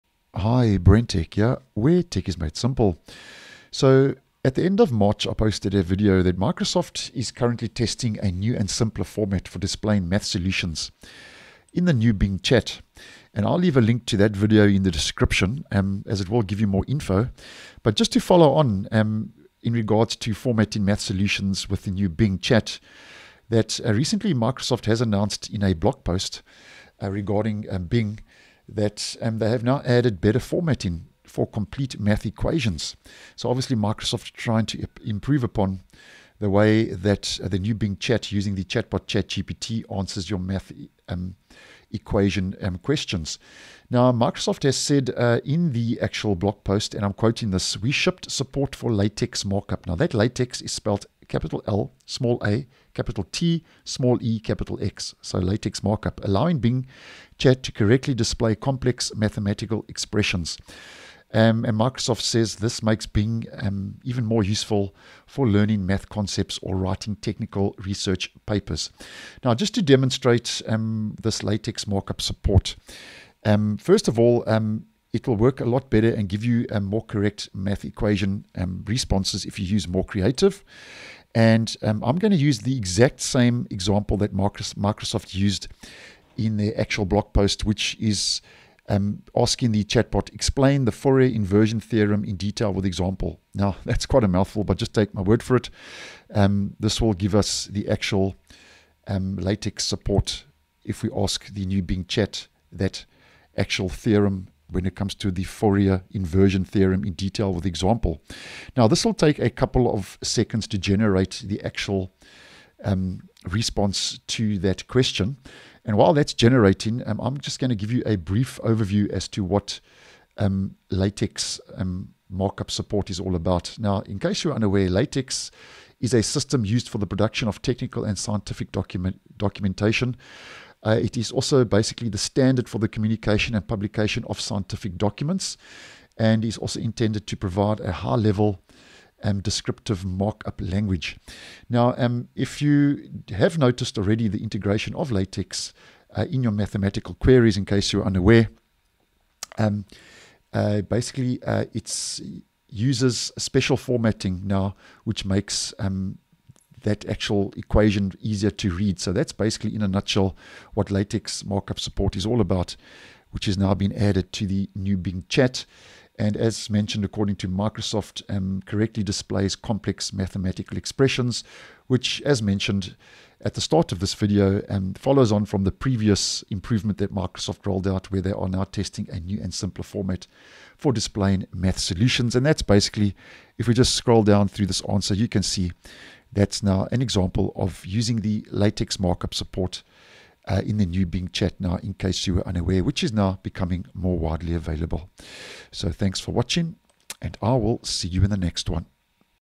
UPDATE Bing Chat gets LaTeX support for correctly displaying complex mathematical equations | How to
Microsoft is currently testing a new and simpler format for displaying math solutions in Bing Chat Microsoft has now added better formatting for math equations, called LaTeX markup—allowing Bing chat to correctly display complex mathematical expressions. This makes Bing chat more useful for learning math concepts or writing technical research papers.